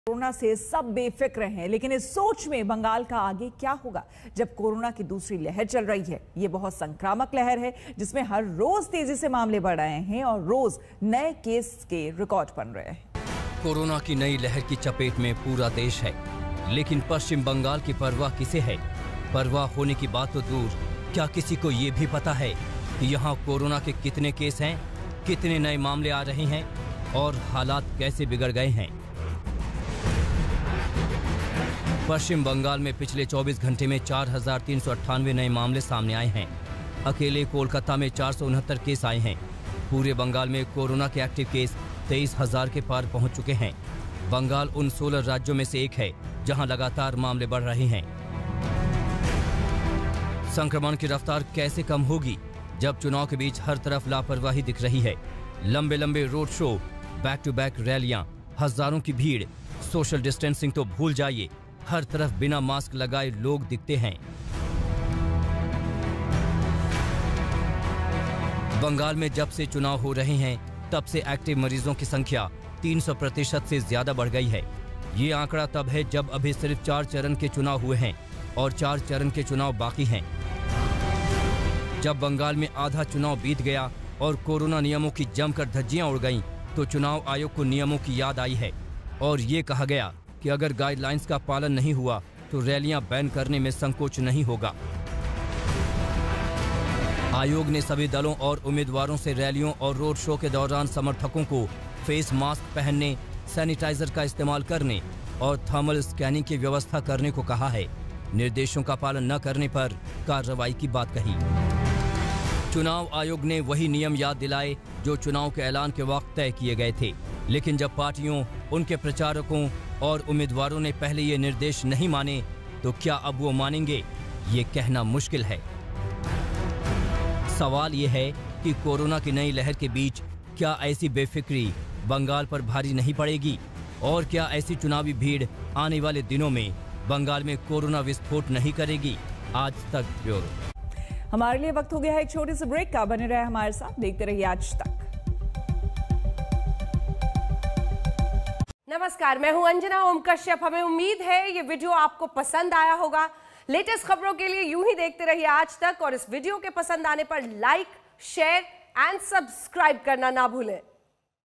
Bengal में रोजाना बढ़ रहे Corona मामले, चुनावी 'लापरवाहियों' के बीच कैसे होगा कंट्रोल !
Bengal में Corona लगातार पांव पसार रहा है. पिछले 24 घंटों में यहां पर नए केस 4000 के पार पहुंचे. इन सबके बावजूद चुनावी रैलियां, रोड शो हो रहे हैं, जिनमें किसी भी Corona Protocol का पालन नहीं हो रहा है.